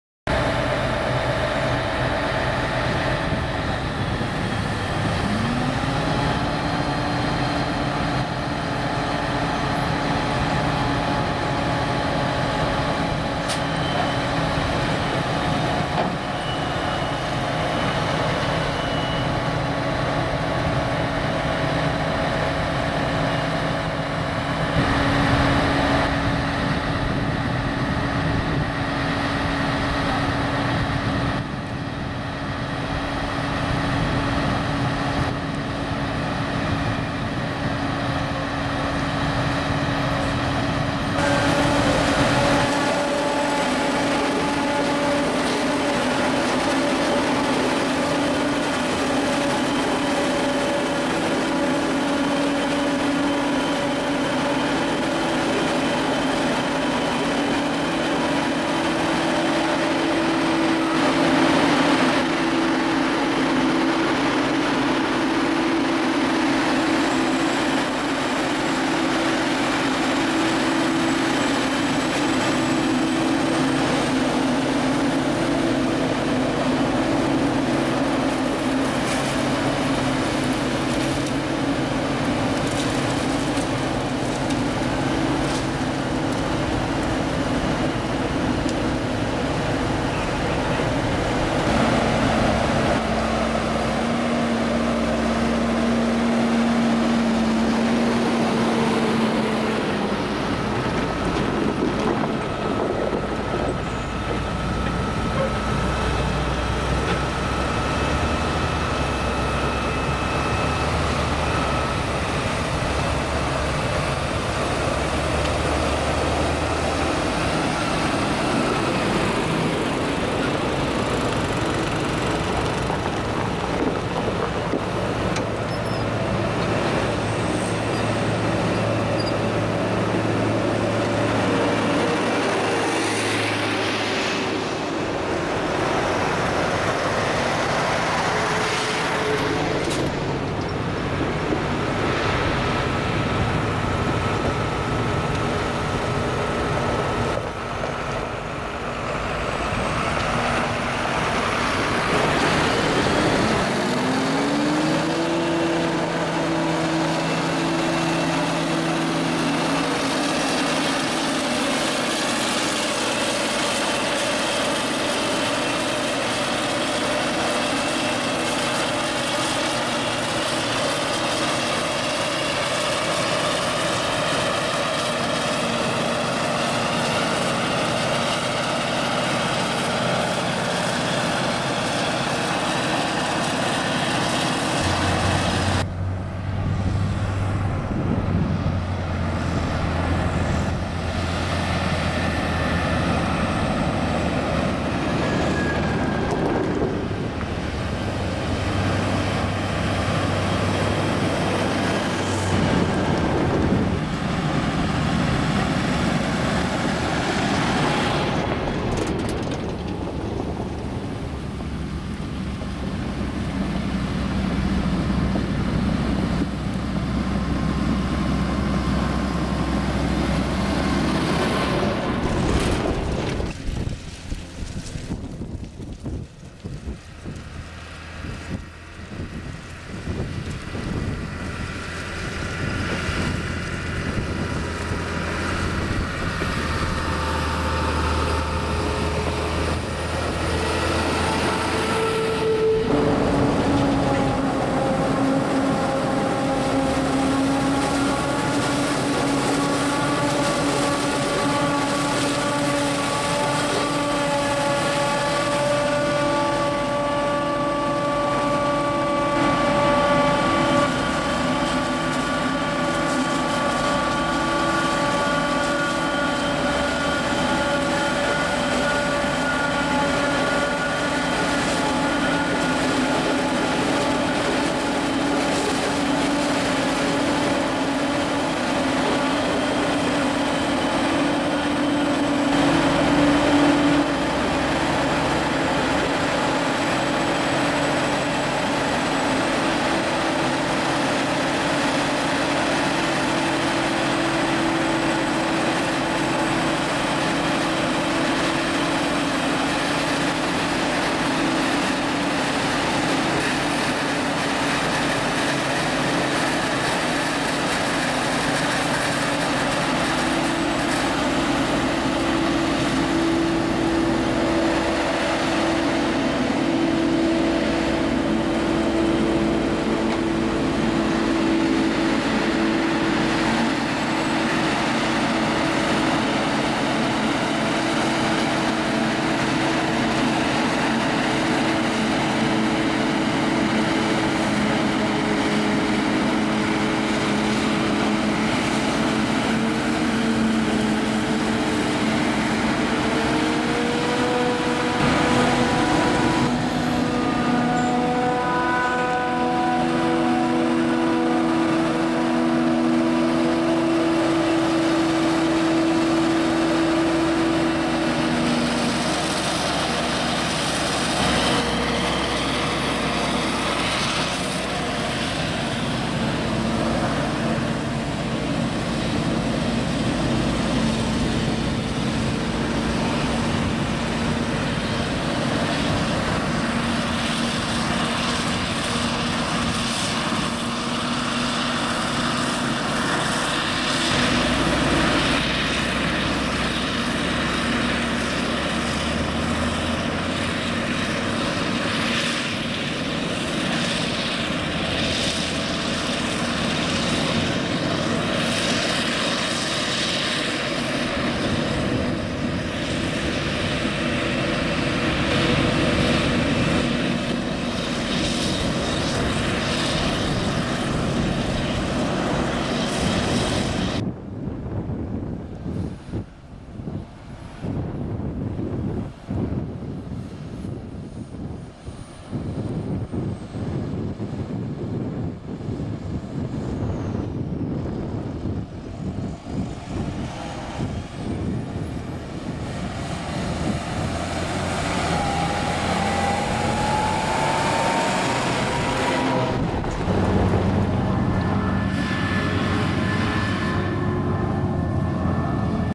Mais Hakselen 2014 : Vital Laschet, Claas Jaguar 960/890 en 14 X Fendt 939 - 824
Mais Hakselen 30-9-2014 : Vital Laschet is aan het hakselen voor een Bio-gas installatie. met 2 Claas Jaguars, een 960 met 10 rijen bek en een Jaguar 890 met 8 rijen. Ook rijden ze vanwege de afstand die ze afleggen met 14 combinaties Fendt, varierend van 939,936 tot en met 824, en diverse Schuitemaker en Joskin silagewagens.